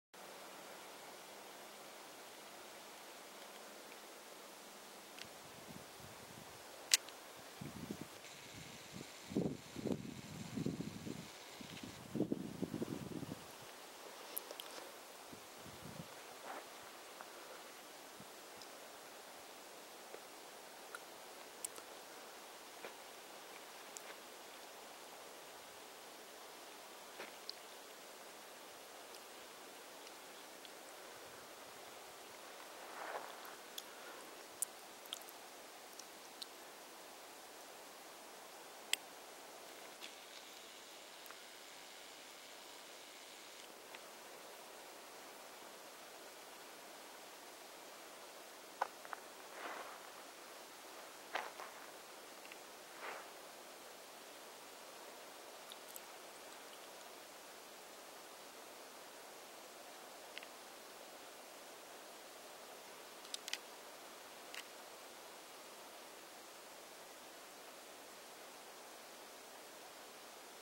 Clouds rolling in on Coldwater Peak summit
This was taken from the summit of Coldwater Peak, in the Mount Margaret Backcountry near Mt. St. Helens. As you can see, the clouds were a constant presence up there!